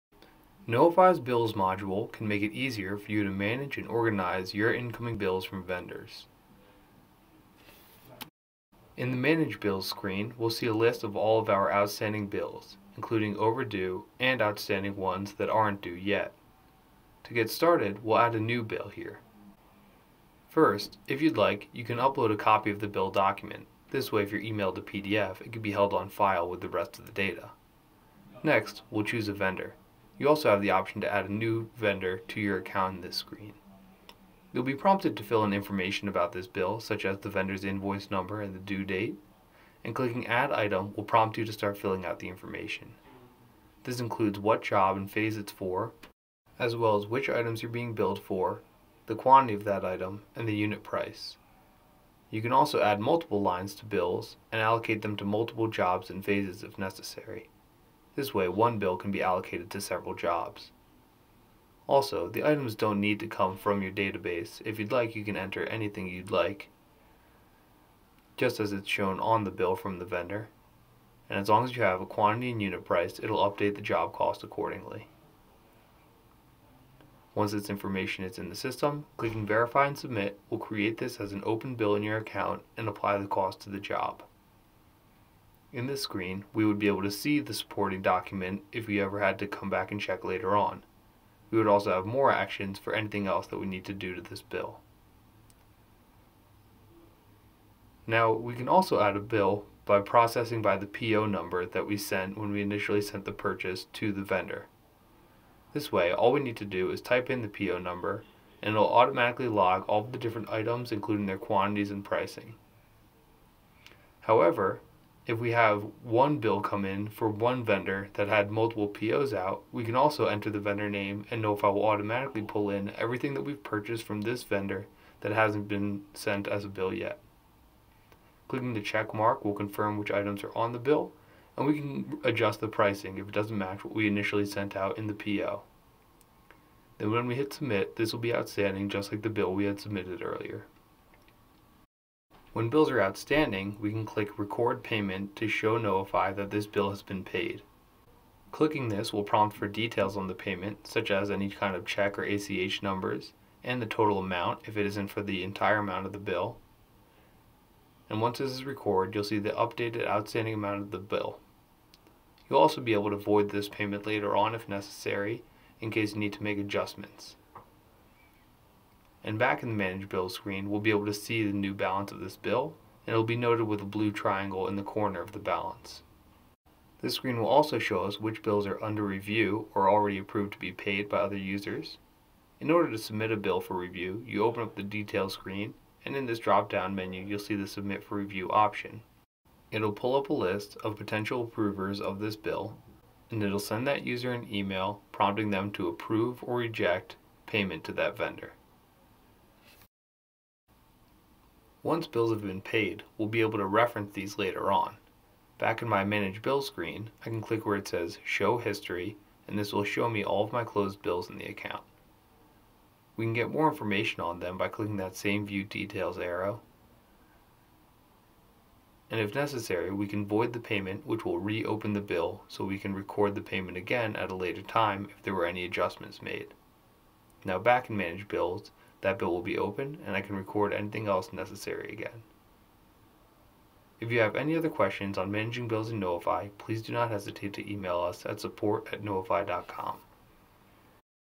Managing bills | Knowify
This video covers how to manage your vendor bills in Knowify.